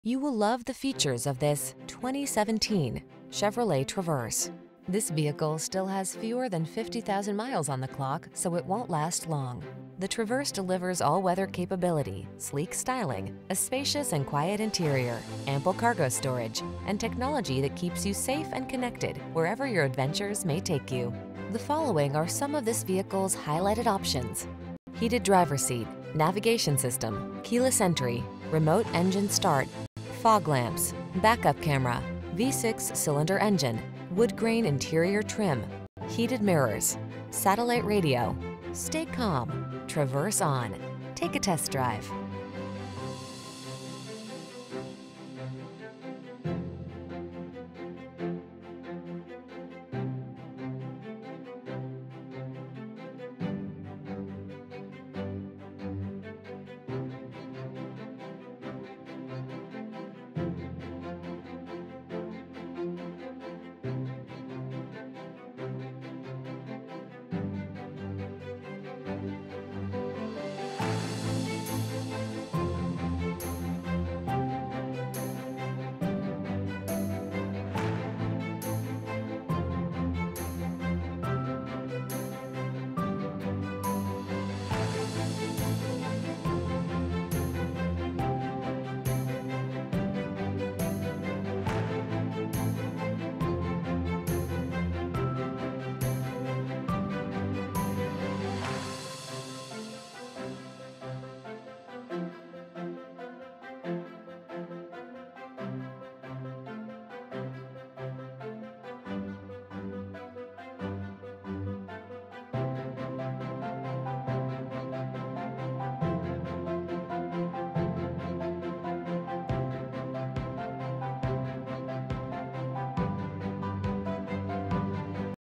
2017 Chevrolet Traverse Owasso, Tulsa, Claremore, Pryor, Broken Arrow, OK N807402
Tungsten Metallic Used 2017 Chevrolet Traverse available in Owasso, Oklahoma at Suburban Chevrolet. Servicing the Tulsa, Claremore, Pryor, Broken Arrow, OK area.

Suburban Chevrolet
1300 S Lynn Riggs Blvd

Odometer is 36908 miles below market average!Tungsten Metallic Traverse LT 1LT 3.6L V6 SIDI FWDAwards:* 2017 KBB.com 10 Most Awarded BrandsReviews:* Smooth ride even when the road isn't; can accommodate up to eight passengers; plenty of cargo space behind the third row; peace of mind that comes with top safety scores. Source: Edmunds